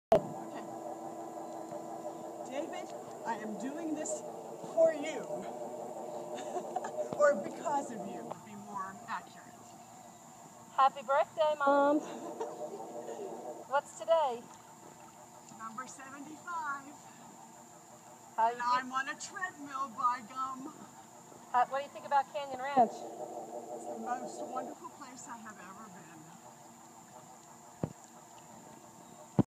Aqua Treadmill HD version
Honey Vickers celebrates her 75th birthday at Canyon Ranch in Tucson, Arizona.